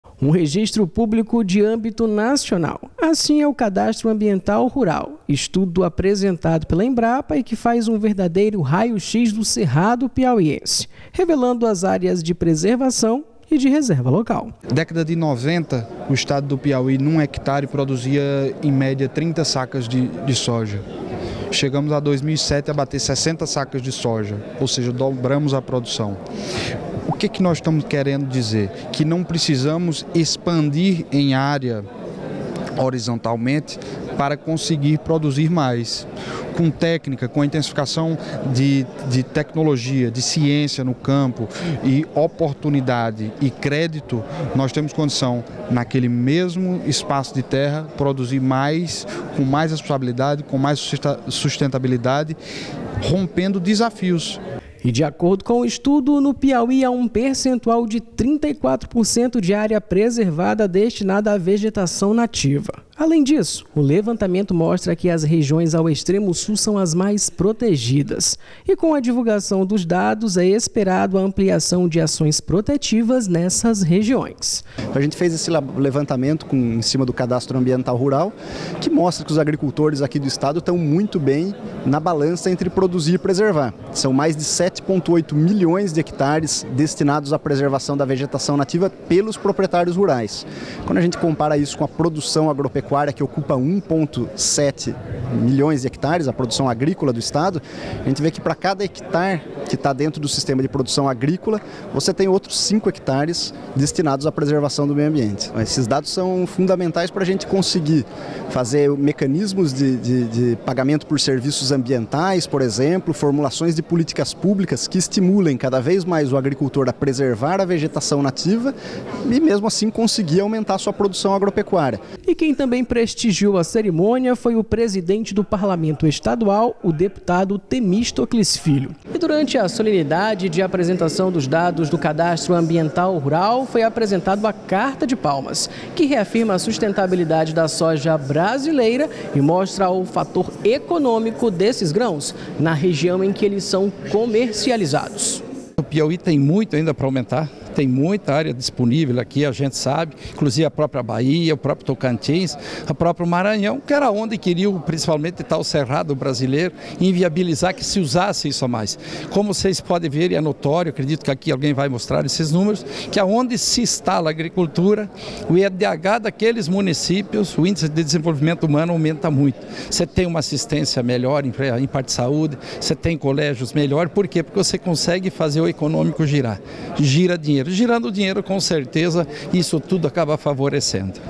Embrapa divulga análise territorial do Cadastro Ambiental Rural do Piauí
A pedido da Associação dos Produtores de Soja do Piauí, a Embrapa divulgou o estudo da análise territorial do cadastro ambiental rural do Piauí. De acordo com o estudo, o Piauí tem um percentual de 34% de área preservada de vegetação nativa.